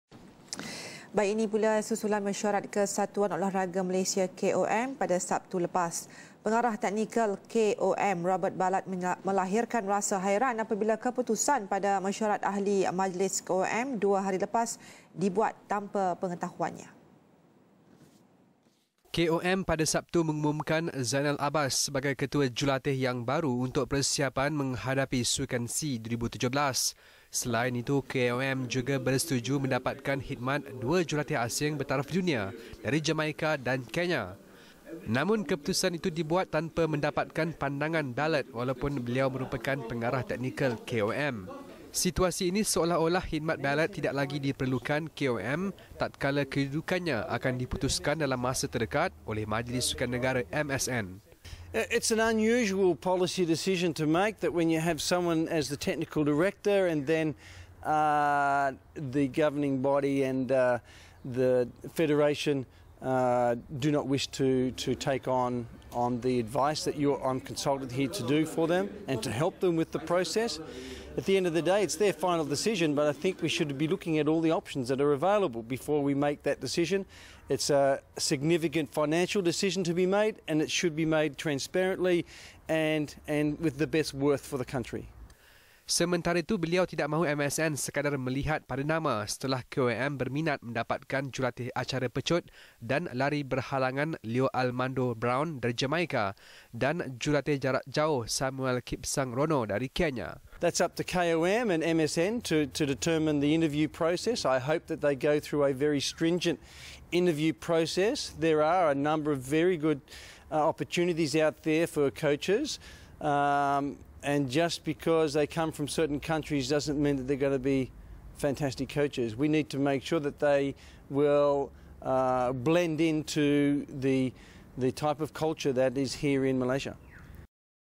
Hasil mesyuarat ahli majlis KOM dipertikai
Pengarah Teknikal KOM, Robert Ballard melahirkan rasa hairan apabila keputusan pada mesyuarat ahli majlis KOM dua hari lepas dibuat tanpa pengetahuanya.